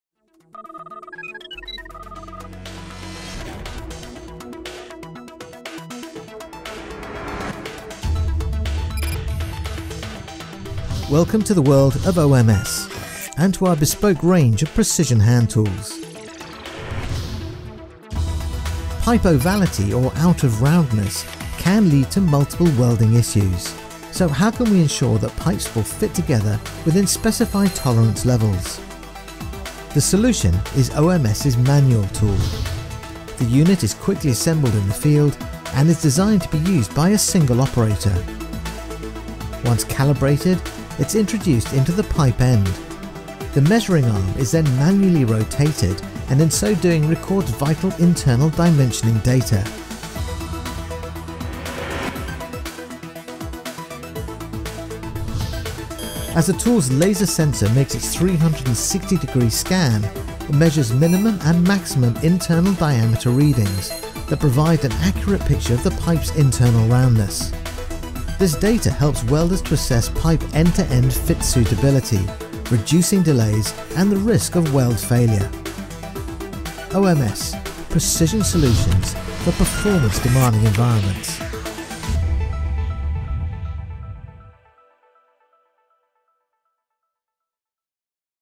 Manual Tool Quality Assurance Hand Tool for precision oil and gas pipeline measurement
Pipe ovality or “out of roundness” can lead to multiple welding issues. So how can we ensure that pipes will fit together within specified tolerance levels?

The solution is OMS’ manual tool.

The unit is easily assembled in the field and is designed to be used by a single operator.

Once calibrated it is introduced into the pipe end. The measuring arm is then manually rotated and in so doing records vital internal dimensioning data.

As the tools’ laser sensor makes its 360 degree scan, it measures minimum and maximum internal diameter readings that provide an accurate picture of the pipe’s internal roundness.  This data helps welders to assess pipe end to end fit suitability reducing delays and the risk of weld failure.

OMS – supplying performance enhancing solutions.

Precision solutions for performance demanding environments.